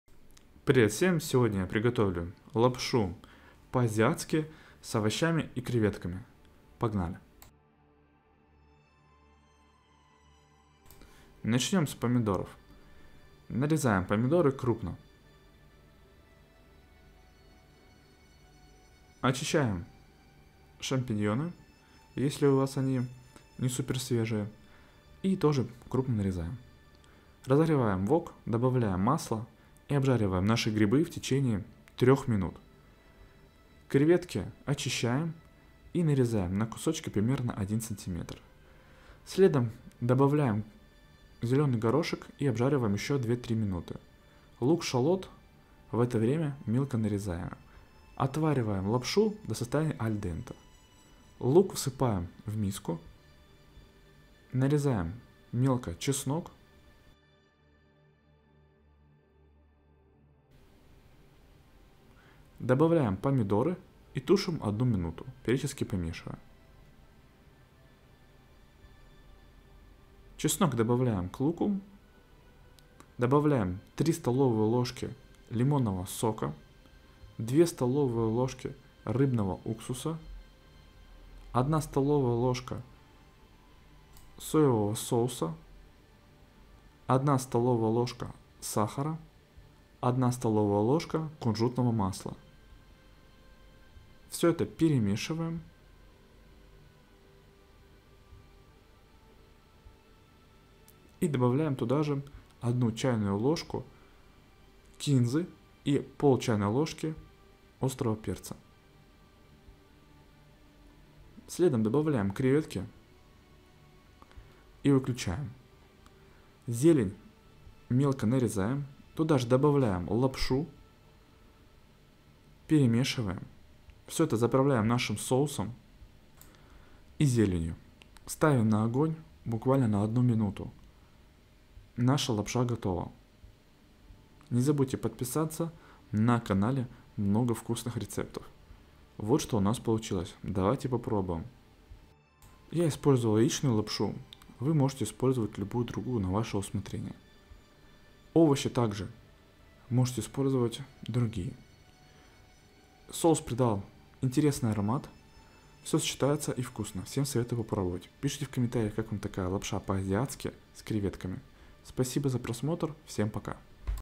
Лапша по Азиатски с Овощами и Креветками
Привет всем! Сегодня я приготовлю лапшу по азиатски с овощами и креветками, а изюминкой станет соус.

Нам понадобится:

Креветки в очищенном виде 150 гр
Лук шалот 1 шт
Лимонный сок 3 ст.л
Горошек замороженный 100 гр
Помидор 1 шт
Лапша яичная 200 гр
Грибы 200 гр
Сахар 1 ст.л
Кунжутное масло 1 ст.л
Соевый соус 1 ст.л
Рыбный соус 2ст.л
Чеснок 2 зубчика.
Чили 0.5 ч.л
Кинза сушенная 1 ч.л
Зелень

Приготовление:

1. Нарезаем помидор крупно, нарезаем мелко грибы.
2. Разогреваем вок сковороду, добавляем подсолнечное масло, обжариваем грибы 3 минут.
3. Креветки очищаем и нарезаем на кусочки примерно 1 см.
4. К грибам добавляем зеленый горошек, обжариваем ещё 2-3 минуты.
5. Лук-шалот мелко нарезаем всыпаем в миску отдельную.
6. Отвариваем лапшу до состояние аль денте.
7. Чеснок мелко нарезаем, добавляем к луку.
8. К грибам с горошком добавляем помидоры и тушим ещё одну минуту.
9. К луку добавляем 3 ст.л сока, 2 ст.л рыбного соуса, 1 ст.л соевого соуса, 1 ст.л  сахара, 1 ст.л кунжутного масла, 1 ч.л кинзы, 0.5 ч.л острого перца  всё это перемешиваем.
10. Креветки добавляем к грибам и выключаем.
11. Туда же добавляем лапшу, зелень мелко нарезаем и добавляем туда же, заливаем соусом, ставим на огонь на 1 минуту перемешиваем.

Приятного аппетита!